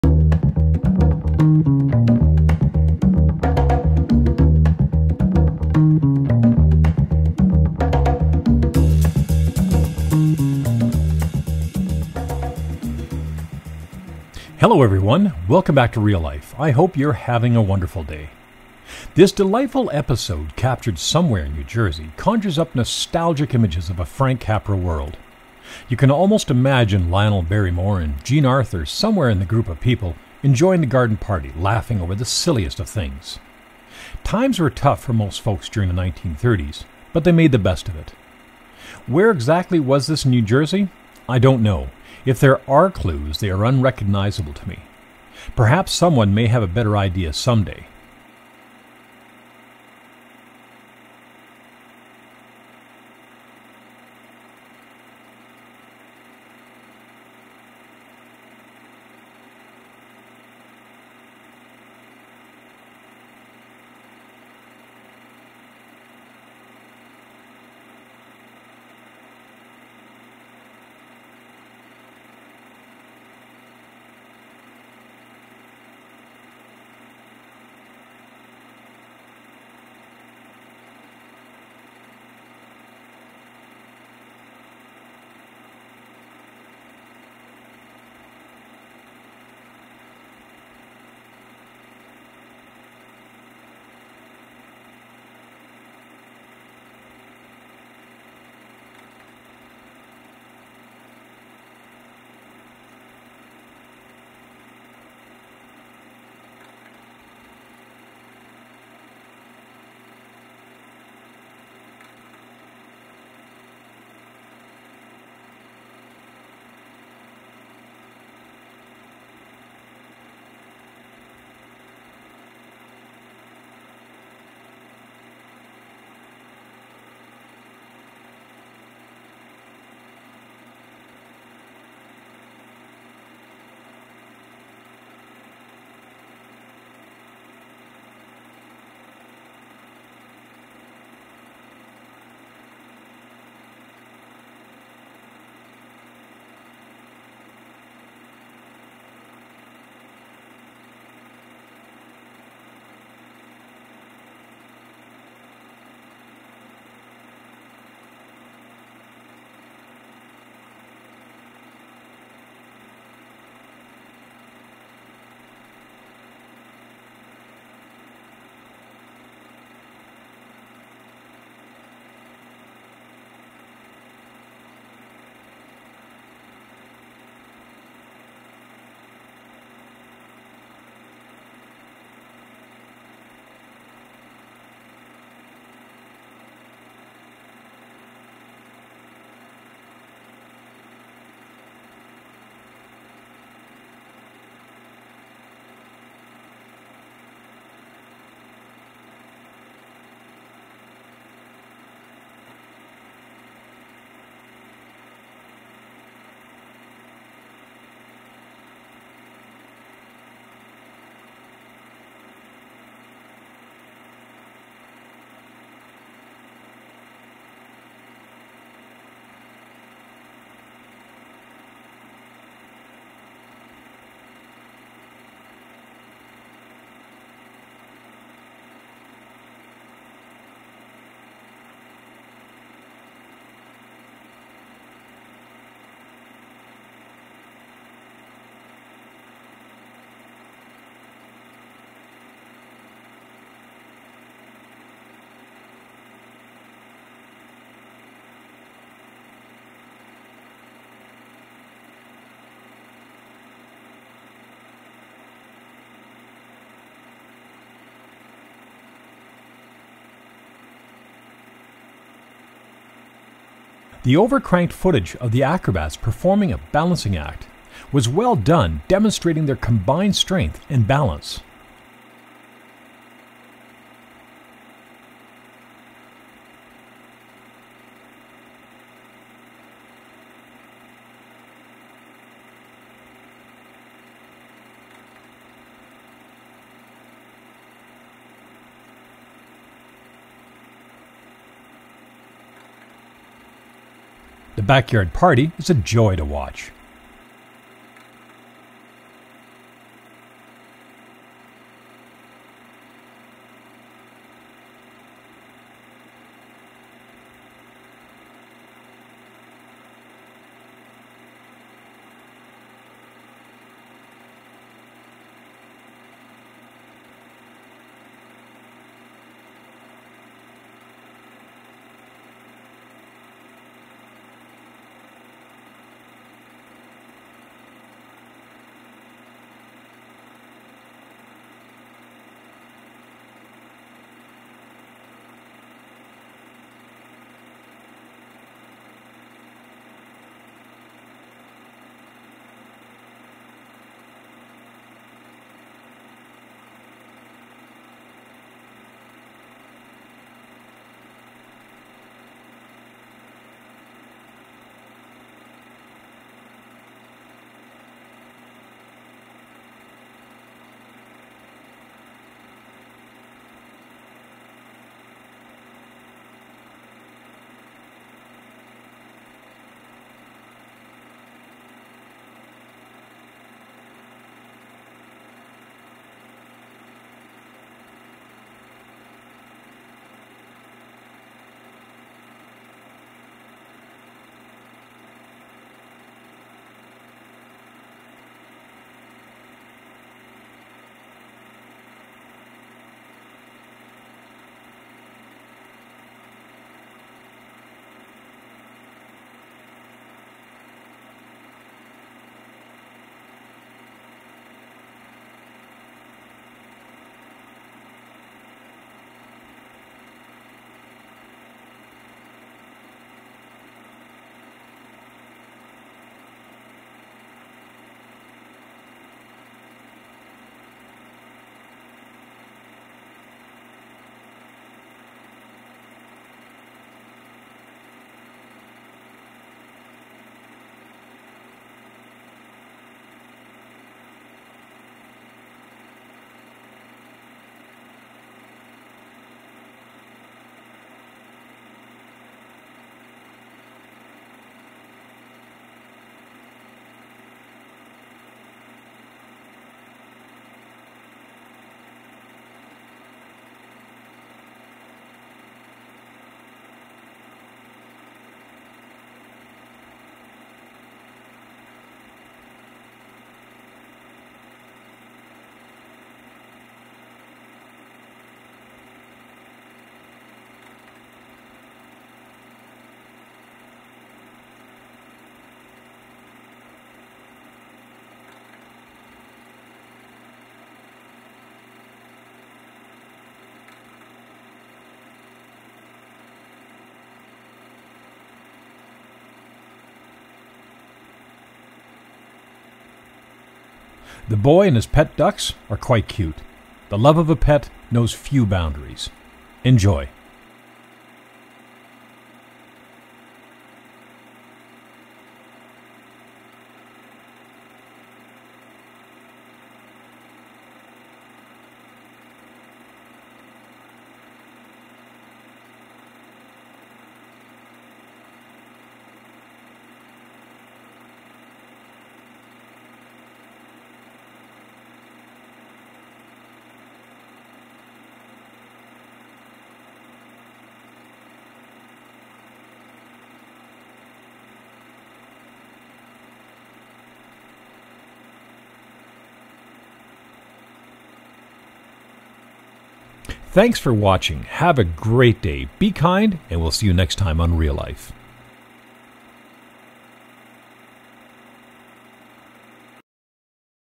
1935 Life during the Great Depression, people kids, acrobats, backyard games, New Jersey
1935 Life during the Great Depression, people kids, acrobats, backyard games, New Jersey

9 min 06 secs, 16mm 2k digital scan
b&w

80+ hours original programming. Reel Life, PTOCS & CanadaMotorSports

episode 1018